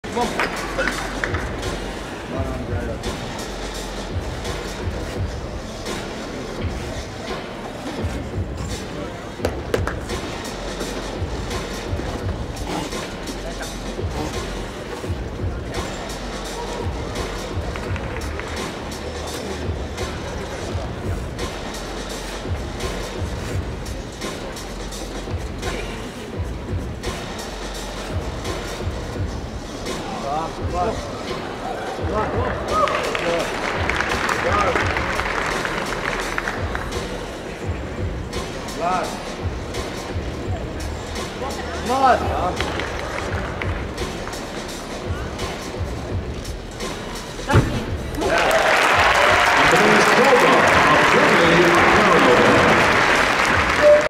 Andreas Toba (GER) - Parallel Bars - 2020 American Cup
March 7, 2020 - Fiserv Forum - Milwaukee, Wis.